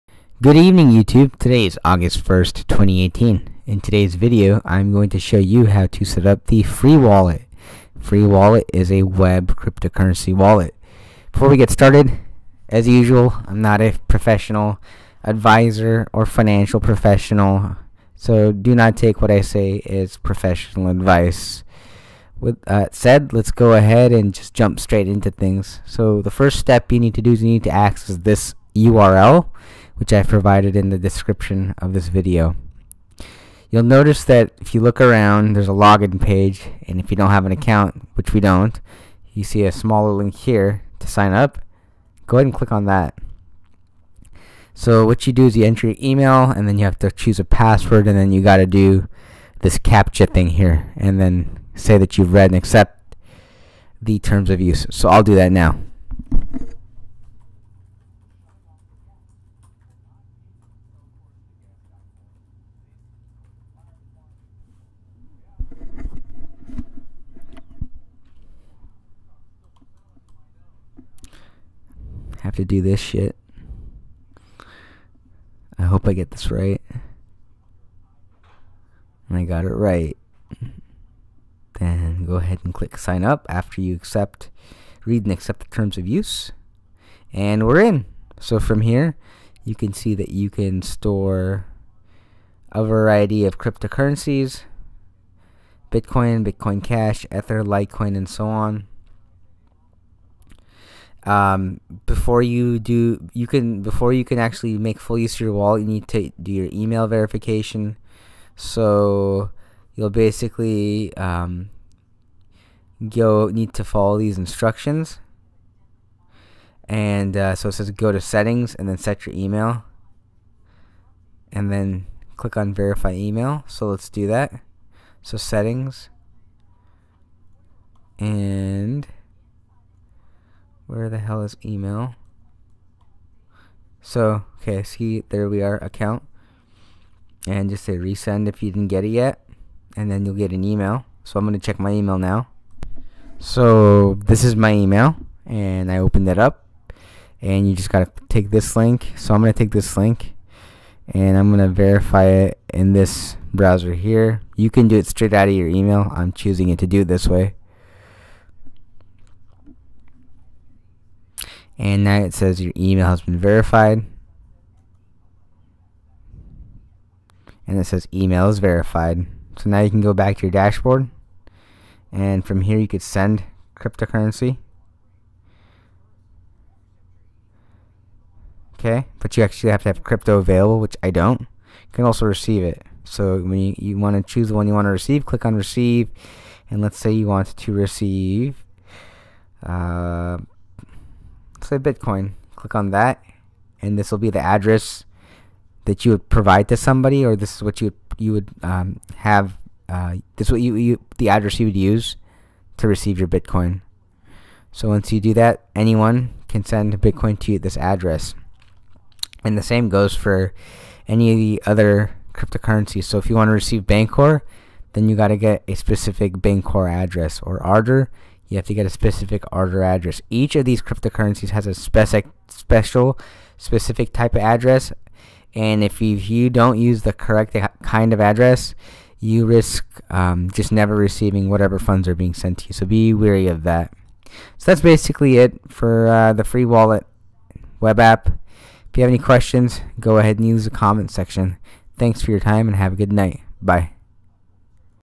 Bitcoin & Monero (XMR & BTC)- Web Wallet Setup - Freewallet
Video Summary:
The purpose of this video is to show you how to quickly and easily create a wallet for storing your IOTA tokens.

Have questions? Comment them in.